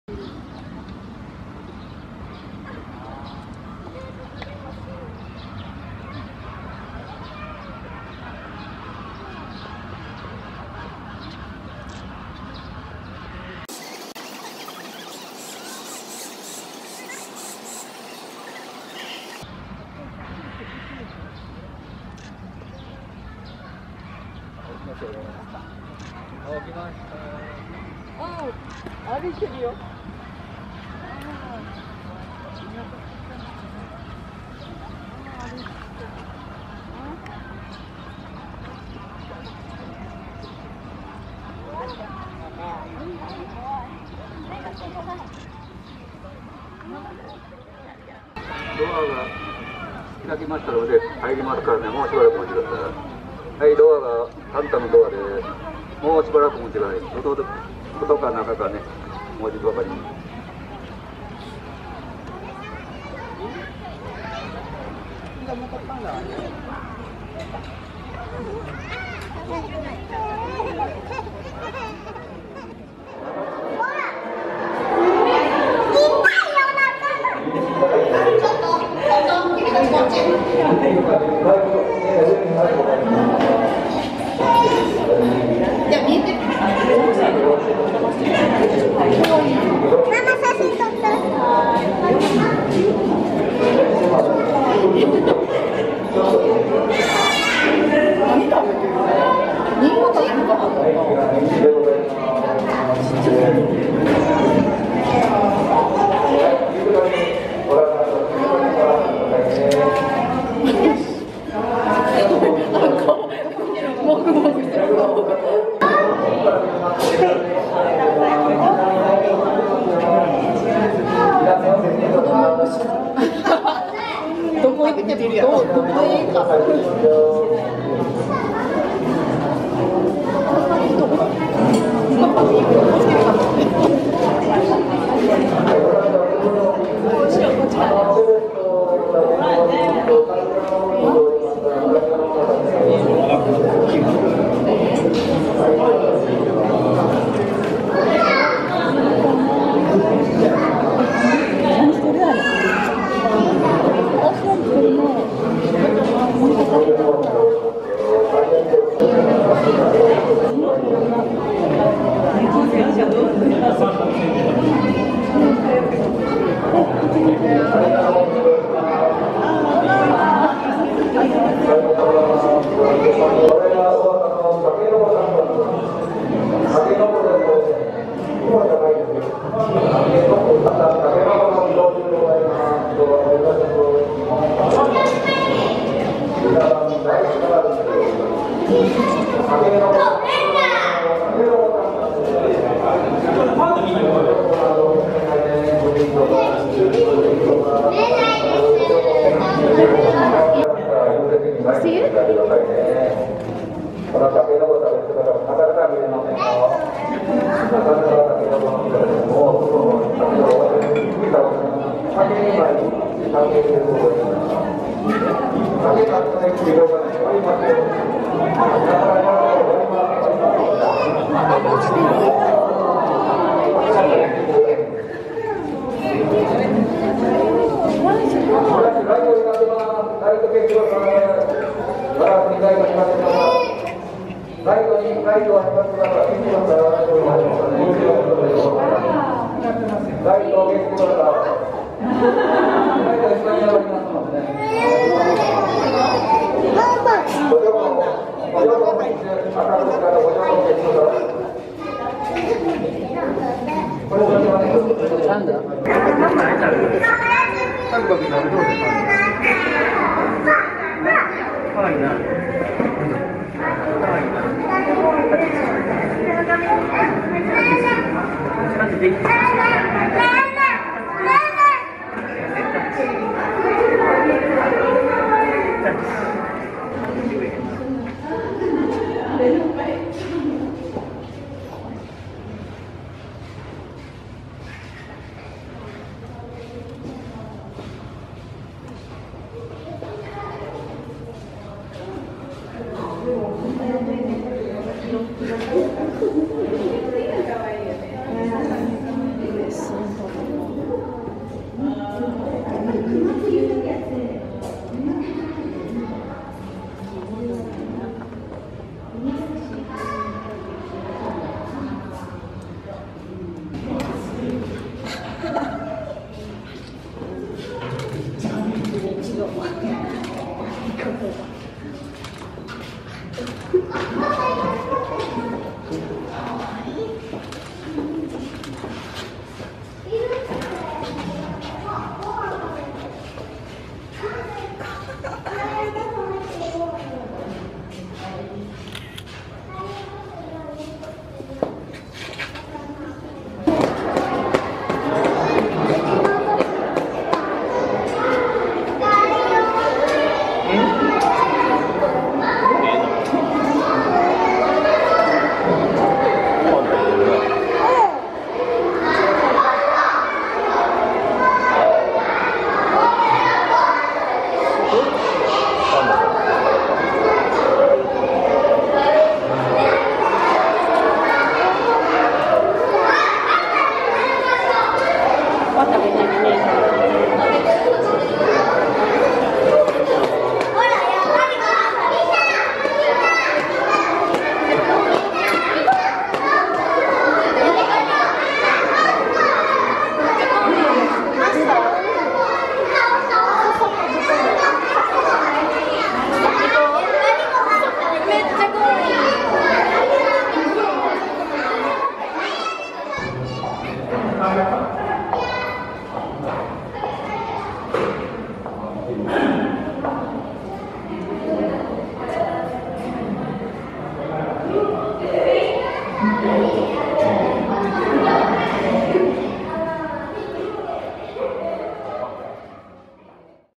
2019年5月27日のパンダのお食事タイム
気温上昇　朝は　寝てました
10時前に　起きて　ウロウロして
室内で　ニンジンや　ペレットのビスケットのおやつを食べて
小さい　タケノコを　2本　それぞれ　両手に持って　食べました。
棒の竹（幹？）　や　笹竹を食べました。
床に這いつくばって　寝てました。